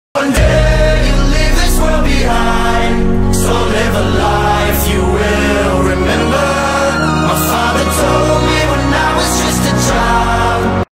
Painting portraits on large canvases. Artist painting process
Elevate your walls with wall art that's both striking and on-trend!